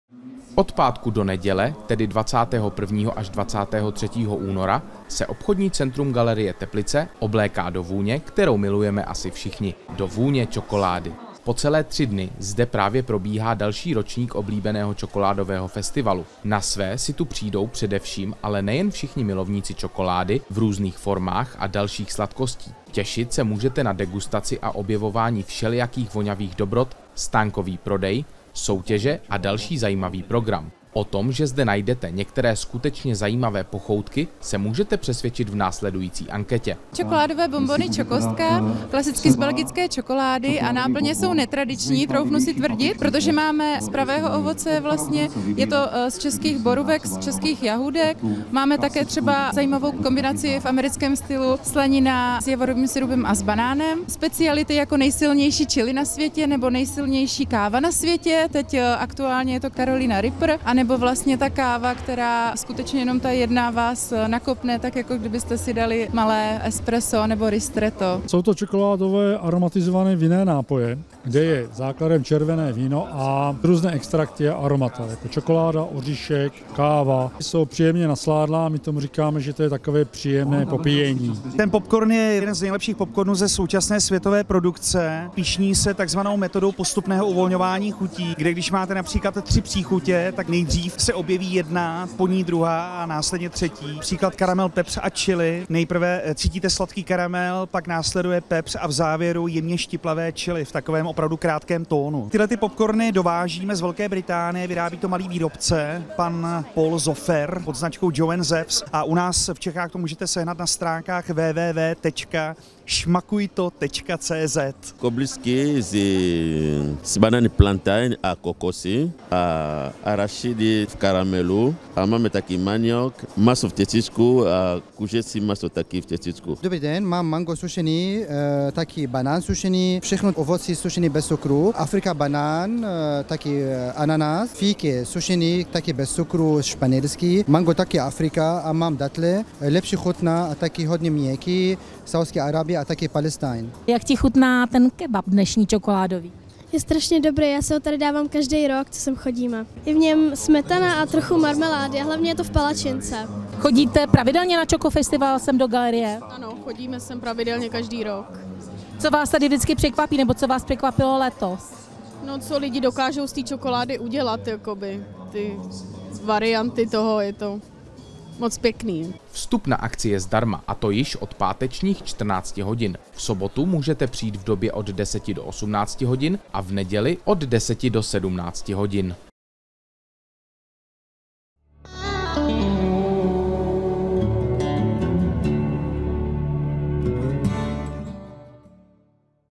Čokoládový festival 2020 Galerie Teplice
Čokoládový festival v Galerii Teplice - rok od roku plnější, barevnější a ještě sladší! Pokud jste ještě na tuto akci, kdy se celé centrum zahalí do omamné vůně čokolády, nezavítali, tak to koukejte letos napravit. Degustace, soutěže, stánkový prodej a mnohem víc 21. až 23. února 2020!

Vstup ZDARMA!